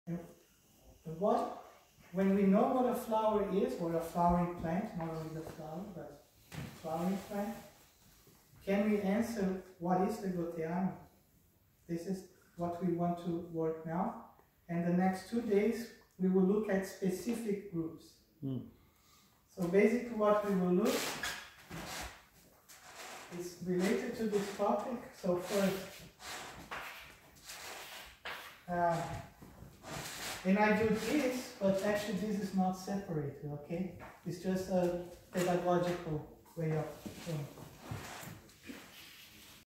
Joao Felipe Ginefa Toni Learning to Understand the Archetypal Plant and Crop Plant
Goetheanum  Glashaus 3 day Course with Professor Toni Feb. 2020
More videos to come from Shabari Bird at Quantum Agriculture